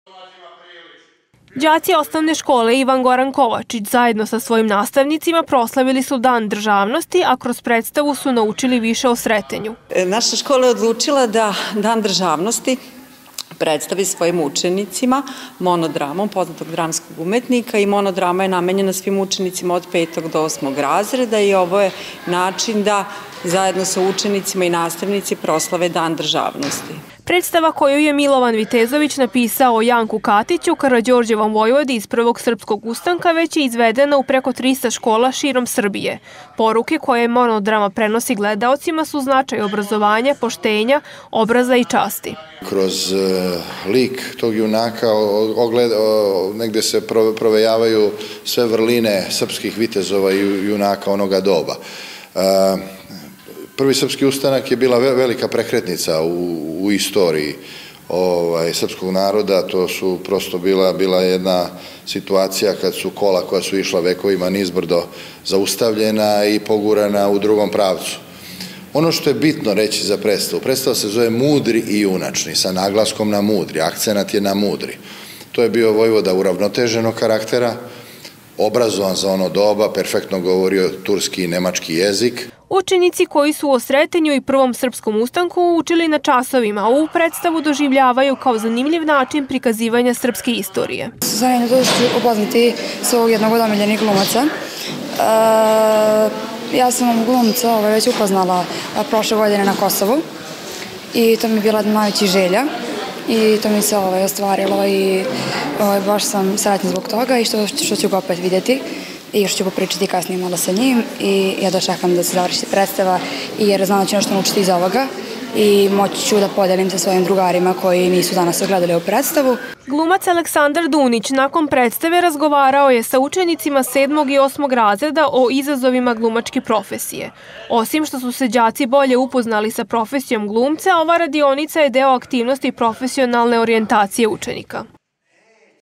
Monodrama o vojvodi Janku Katiću, Karađorđevoj desnoj ruci
Povodom Dana državnosti Republike Srbije u OŠ „Ivan Goran Kovačić“ odigrana je monodrama  u izvođenju dramskog umetnika Aleksandra Dunića. Predstava pod nazivom „Mudri i junačni vojvoda Janko Katić“ rađena je po tekstu Milovana Vitezovića.

Đaci OŠ „Ivan Goran Kovačić“ zajedno sa svojim nastavnicima proslavili su Dan državnosti, a kroz predstavu su naučili više o Sretenju.

Predstava koju je Milovan Vitezović napisao  o Janku Katiću, Karađorđevom vojvodi iz Prvog srpskog ustanka već je izvedena u preko 300 škola širom Srbije. Poruke koje monodrama prenosi gledaocima su značaj obrazovanja,poštenja, obraza i časti.

Učenici koji su o Sretenju i Prvom srpskom ustanku učili na časovima, ovu predstavu doživljavaju kao zanimljiv način prikazivanja srpske istorije.

Glumac Aleksandar Dunić nakon predstave razgovarao je sa učenicima sedmog i osmog razreda o izazovima glumačke profesije. Osim što su se đaci bolje upoznali sa profesijom glumca, ova radionica je deo aktivnosti profesionalne orijentacije učenika.